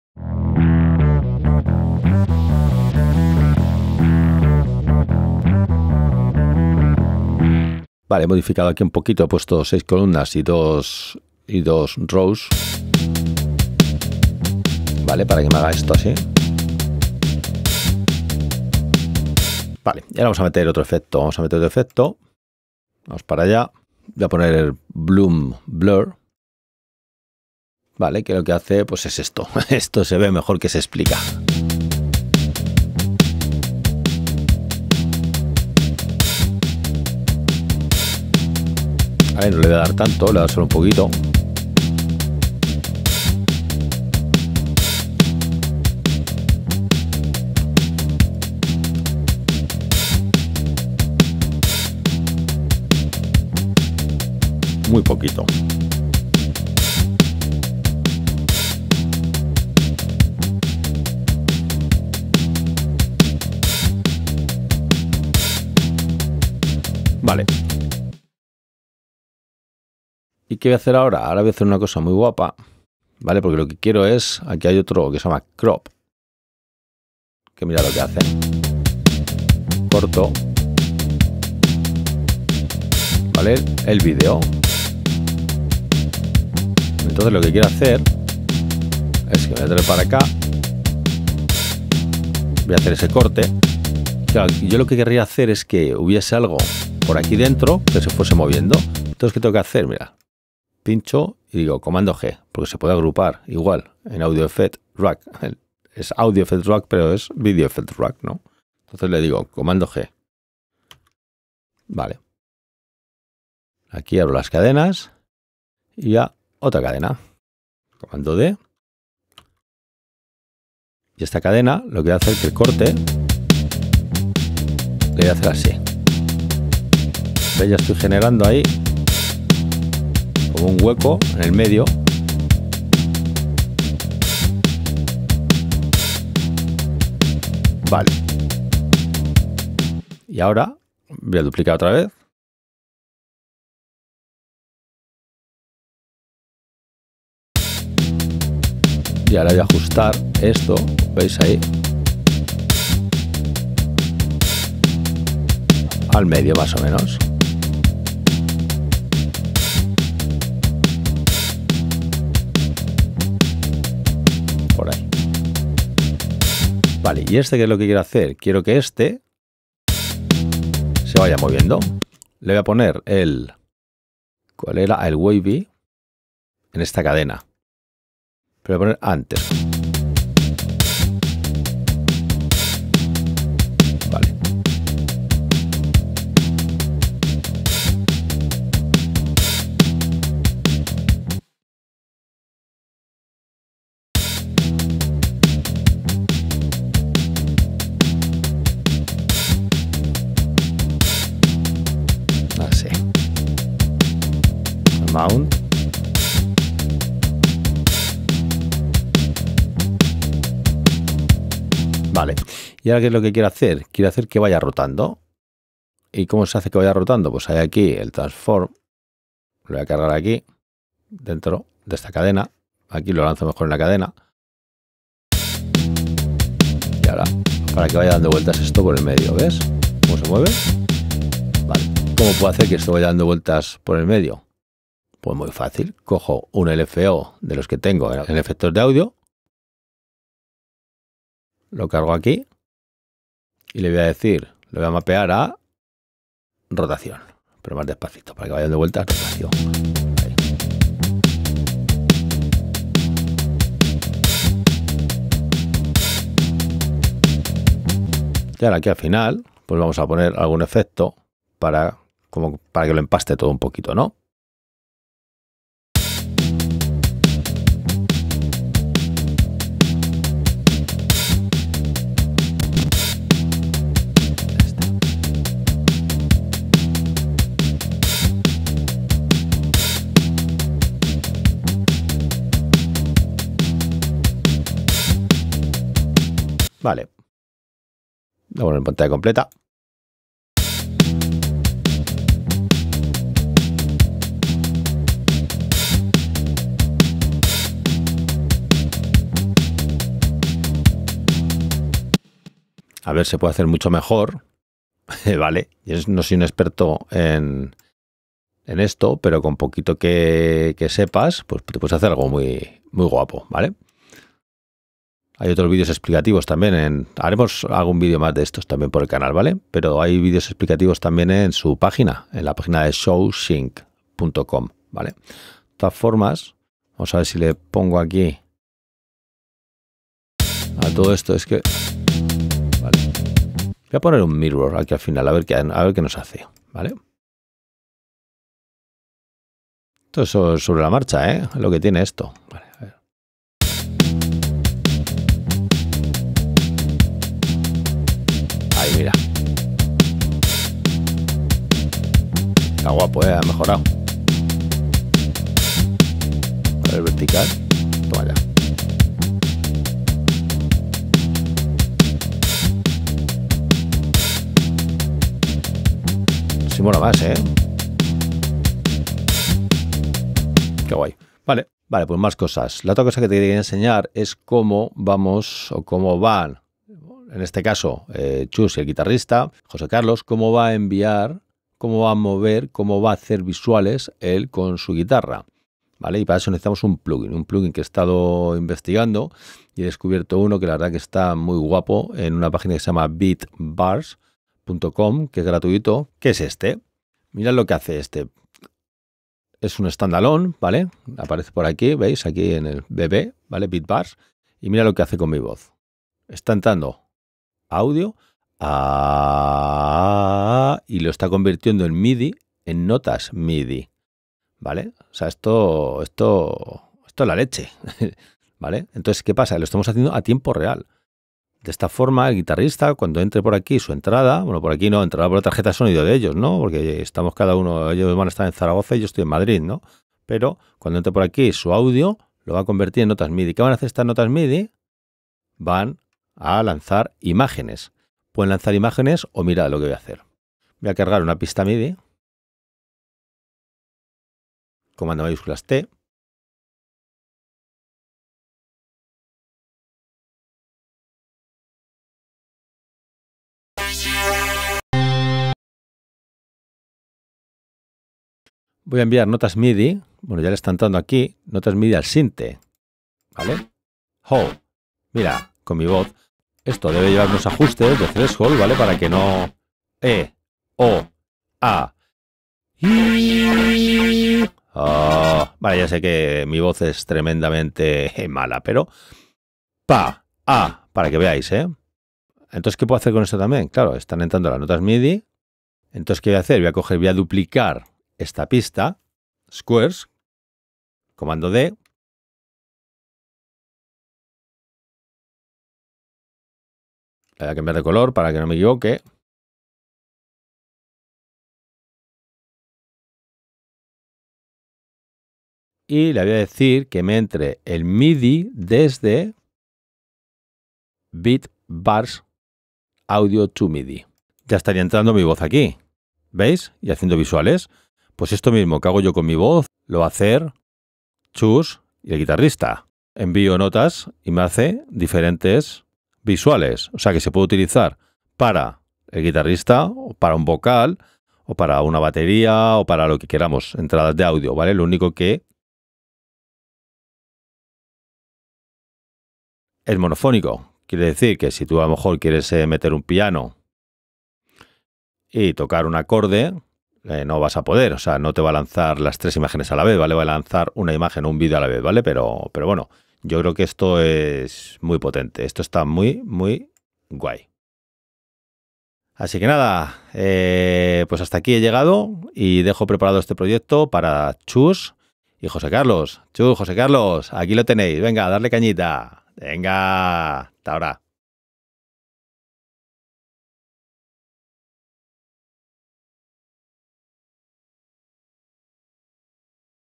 Convierte audio en visuales con César Berti (2ª Parte)
Aprende a convertir audio en visuales con César Berti utilizando Beats Bars A2M y Videosync.